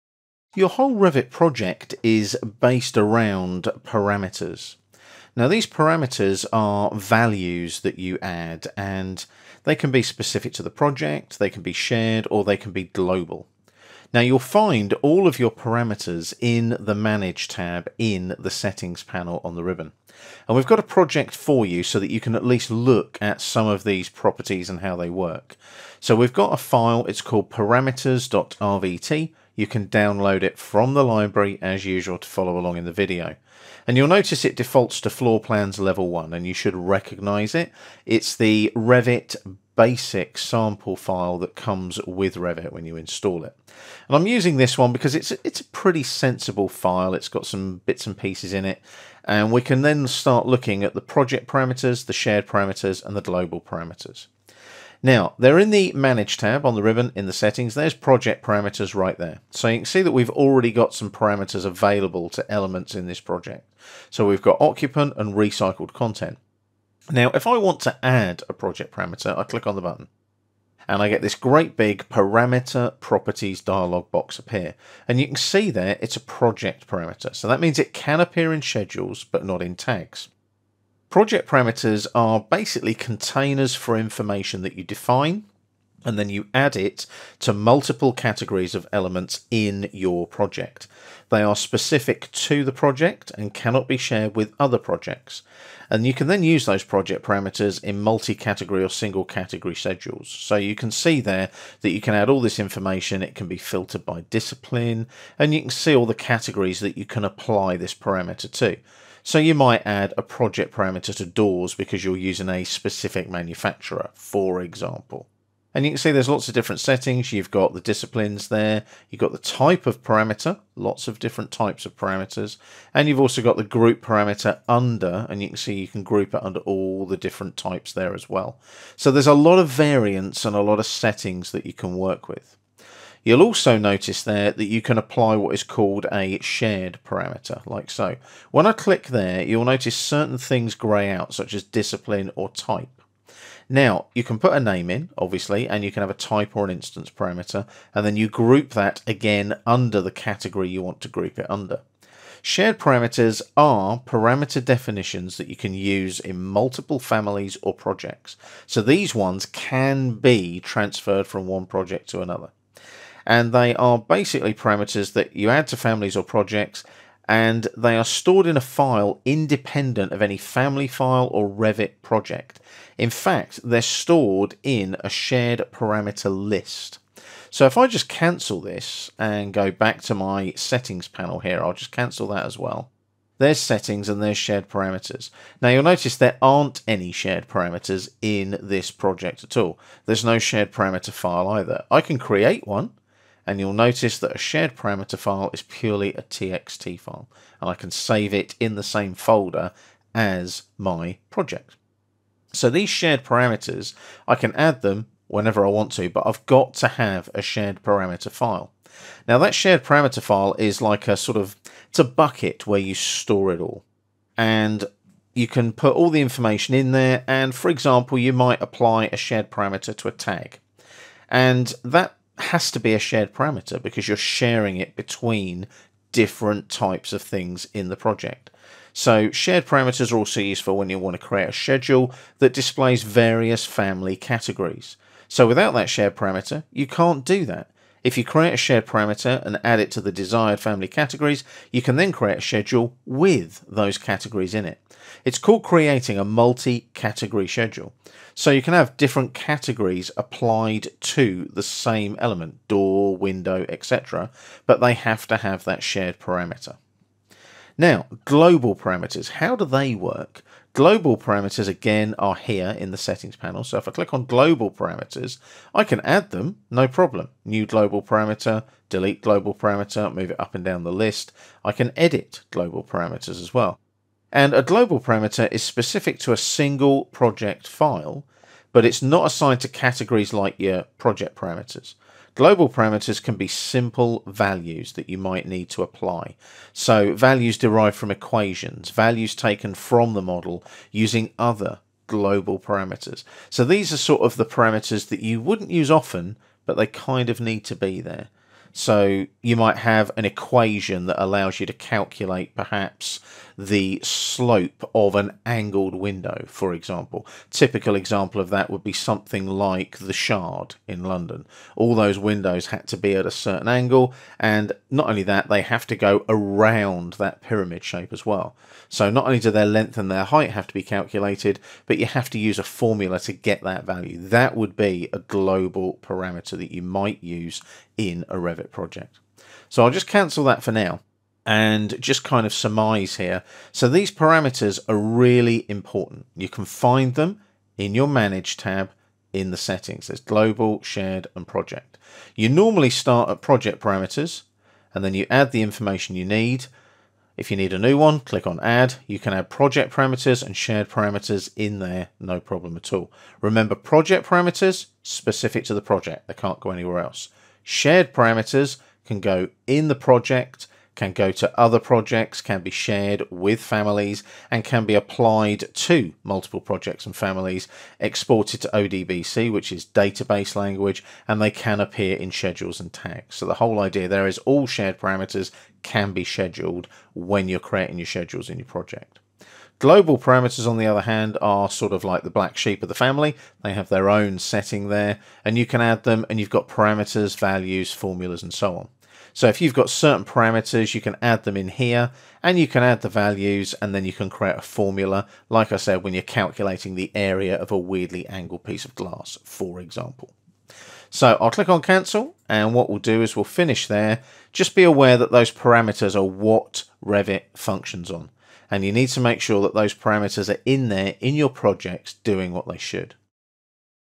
how to Revit Understanding shared, project, and global parameters
Learn how to understand shared, project, and global parameters in Revit MEP. This video will guide you through the basics of each parameter type for efficient project management.
Revit tutorials